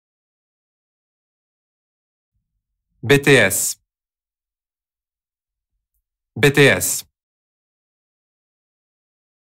How to pronounce BTS in French
This video shows you how to pronounce BTS in French. Speaker has an accent from Amiens (Somme, Hauts-de-France). Collins Dictionary, the home of living language and pioneers of dictionary publishing